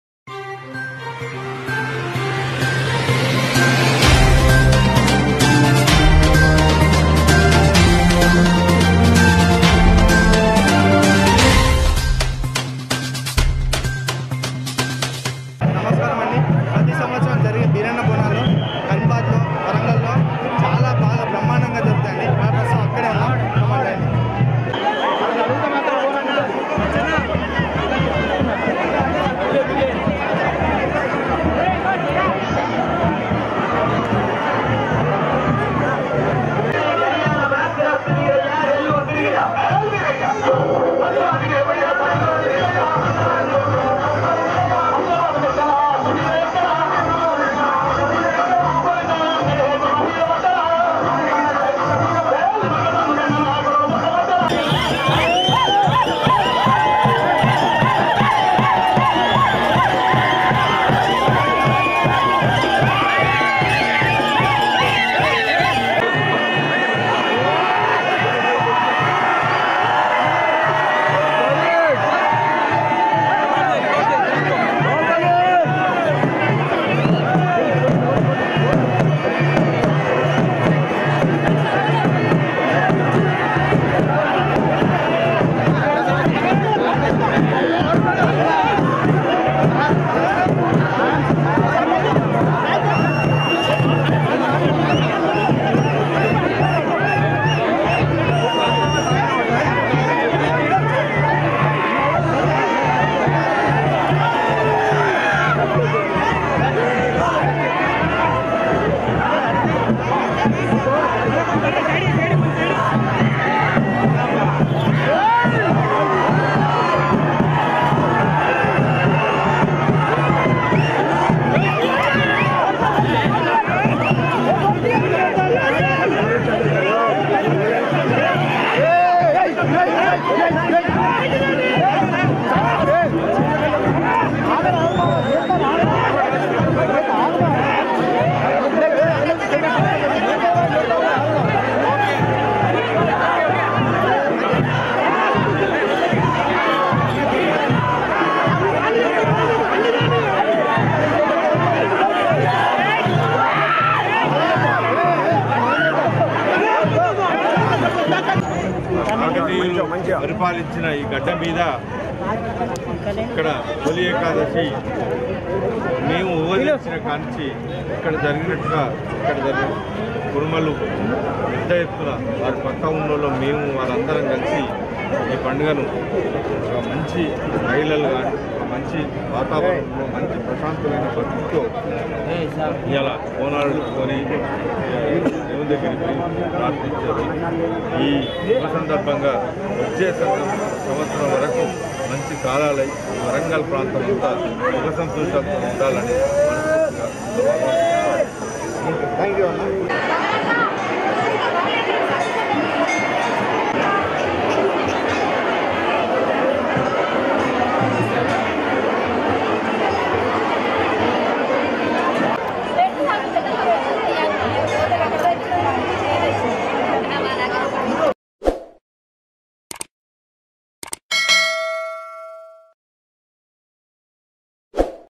వరంగల్ కరీమాబాద్లో వైభవంగా బీరన్న జాతర ll సహకార బ్యాంకు చైర్మన్ ఎర్రబెల్లి ప్రదీప్ రావు ll RK24TV
వరంగల్ ఉర్సు కరీమాబాద్లో అంగరంగ వైభవంగా అంబరాన్ని అంటిన బీరన్న జాతర . జాతర పోటెత్తిన పోటెత్తిన భక్త జనం .యిసికేసిన రాలని జనాల మధ్యలో బీరప్ప జాతర అ అంగరంగ వైభవంగా జరిగింది .చాలా మంది ప్రజలు దాదాపు లక్ష నుంచి రెండు లక్షల మంది జాతరకు విచ్చేసి దేవుడి యొక్క ఆశీర్వాదాలను పొందుకున్నారు ...దీనిలో వరంగల్ పశ్చిమ ఎమ్మెల్యే నన్నపునేని నరేందర్ .మాజీ పార్లమెంటు సభ్యులు సిరిసిల్ల రాజయ్య .మరియు సహకార బ్యాంకు చైర్మన్ ఎర్రబెల్లి ప్రదీప్ రావు. ముఖ్య నాయకులు ప్రజలు పాల్గొన్నారు .అంగరంగ వైభవంగా జరిగిన ఒక వీధి బీరప్ప జాతరలో నృత్యాల తోటి డప్పు వాయిద్యాల తోటి బోనాలను ఆ గుడి దగ్గరికి చేర్చి బీరన్న ఆశీర్వాదాలను పొందుతున్నారు. మీకు ఈ వీడియోలు నచ్చితే చేయండి , మీ COMMENTS ను తెలపండి , ఇంకా ఇలాంటి వీడియోస్ మీరు పొందుటకు మా YOUTUBE ఛానల్ చేసుకోండి